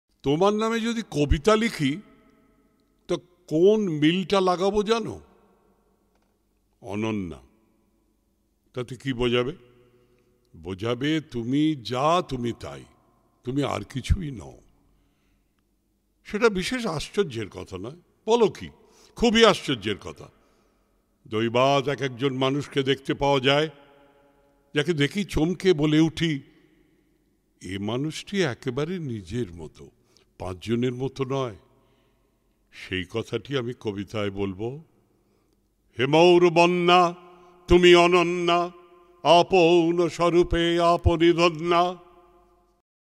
Ananya - Rabindranath Tagore - Suniti Bhose
Ananya - Poet: Rabindranath Tagore - Voice: Suniti Bhose - From Tagore's 'Shesher Kobita' - Painting: Ramananda Bandopadhyay